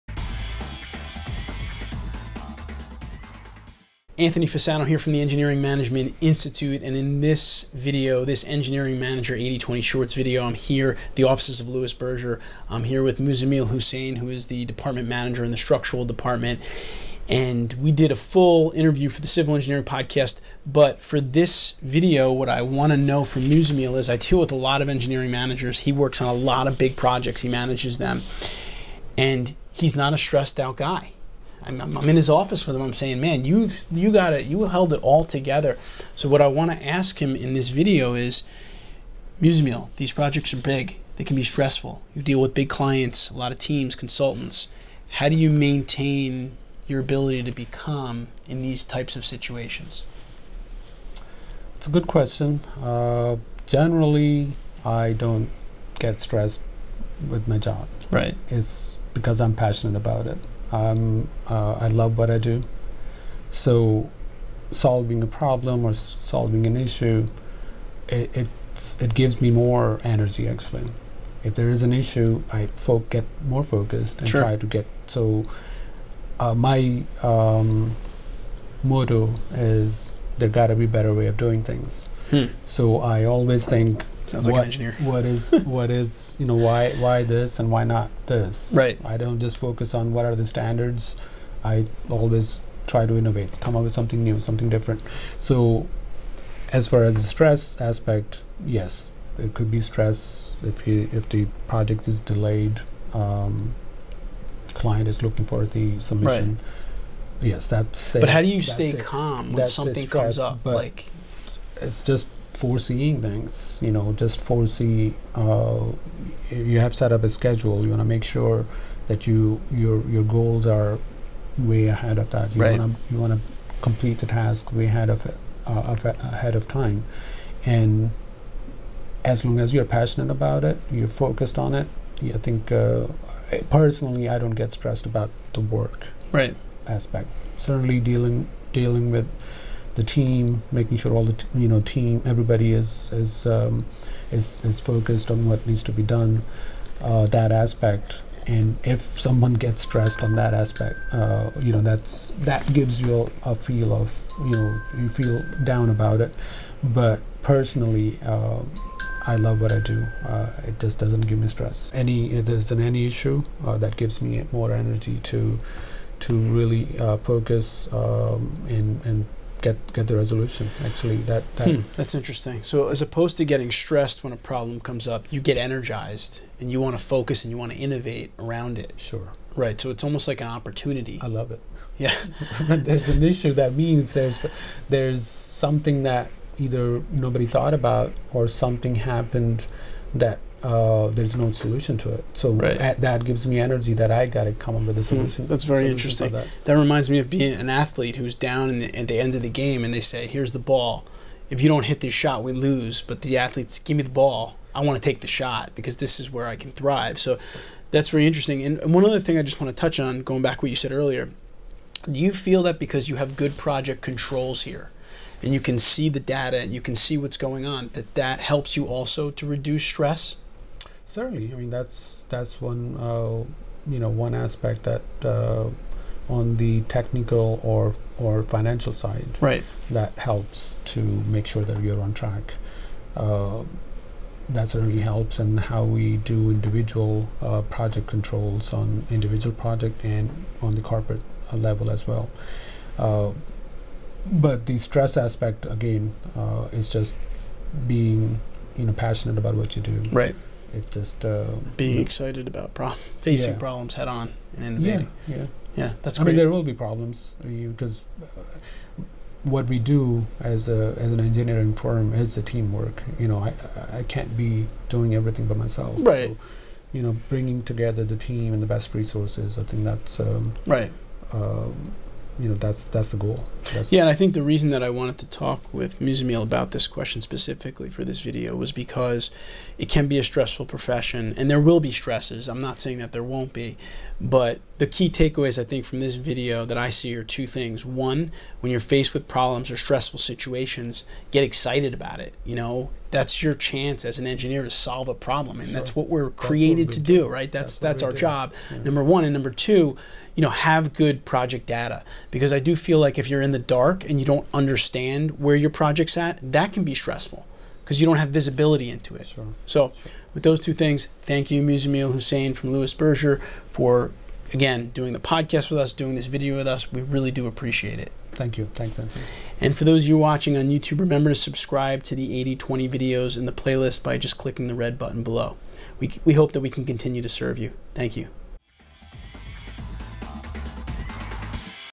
How to Remain Calm in Stressful Engineering Project Situations
In this week's Engineering Manager 80/20 Shorts video, Anthony Fasano, PE of the Engineering Management Institute talks to the Department Manager at Louis Berger, Muzamil Husain, PE about how he remains calm in the face of stressful engineering project situations and how he uses those situations to get innovative and solve problems.